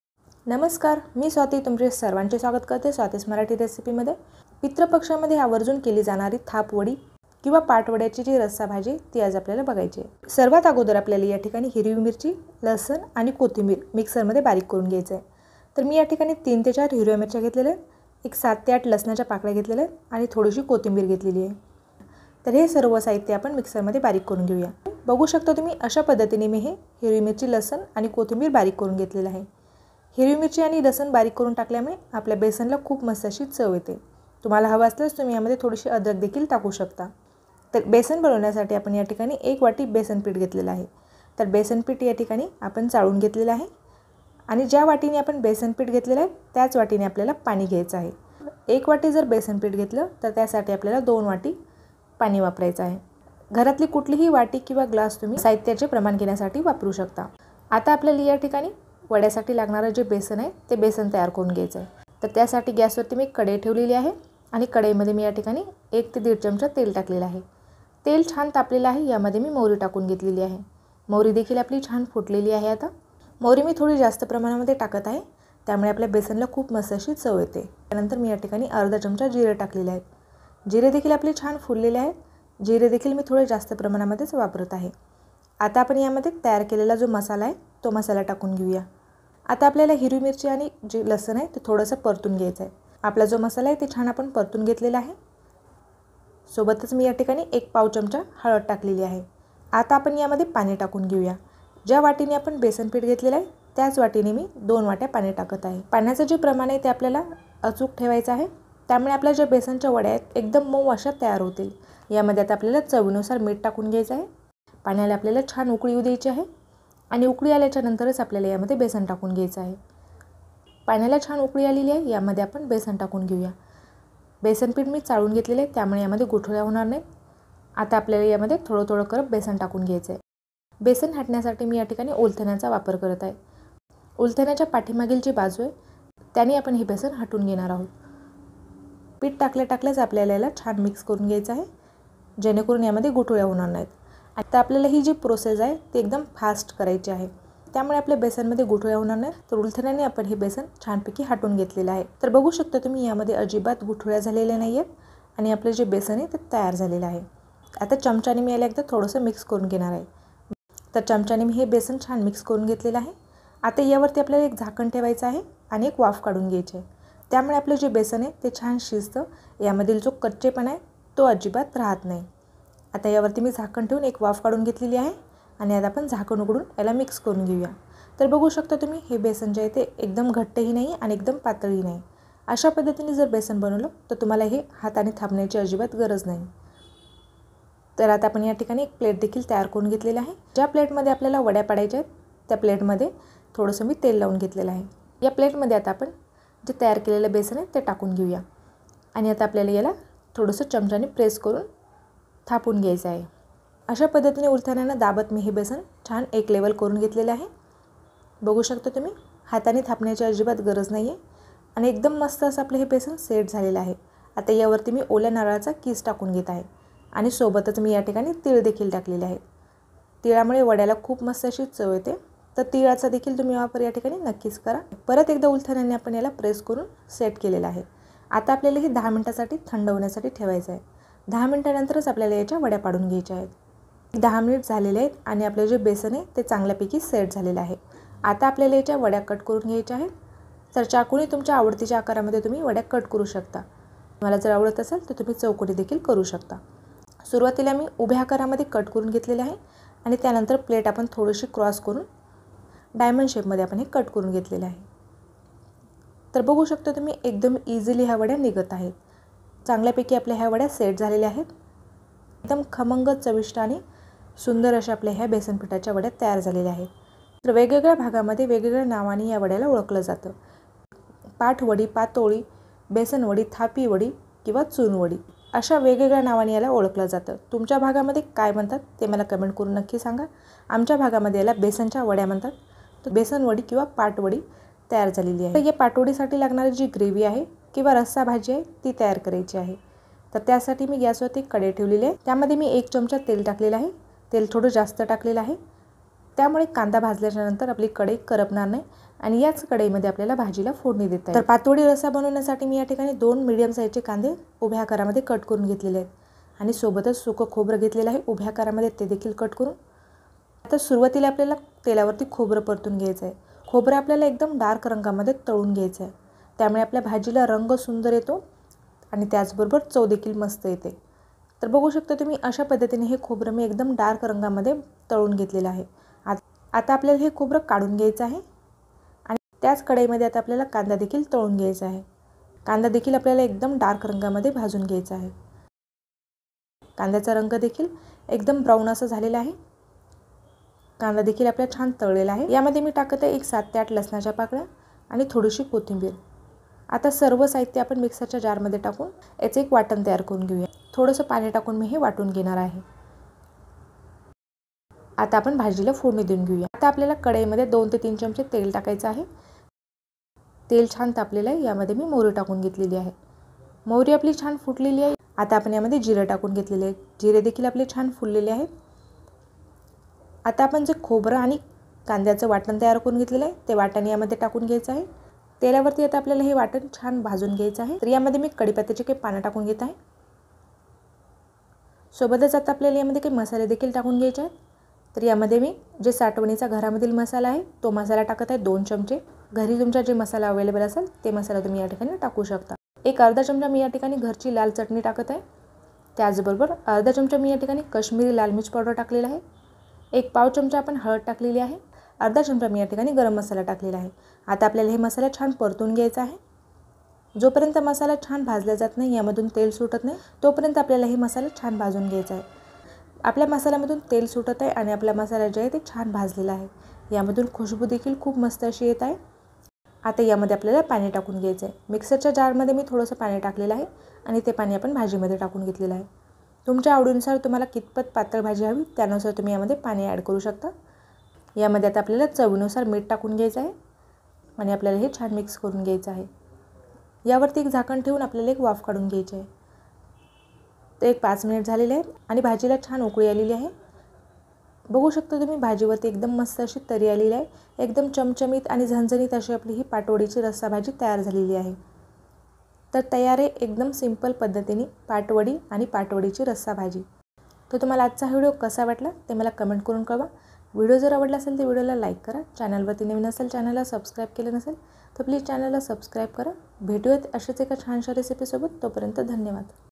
पितृपक्ष विशेष रेसिपी " झणझणीत खमंग पाटवडी/पातोडी | महाराष्ट्राची स्पेशल बेसन वडी रस्सा भाजी Patwadi
बेसन न हटता ,न वडी थापता झटपट पाटवडी रस्सा भाजी | झणझणीत चमचमीत पाटवडी भाजी दोन पोळ्या जास्तच खाल

Key Words Using this video

recipe in hindi, pyaj pata recipe, besan ki sabji bihari, besan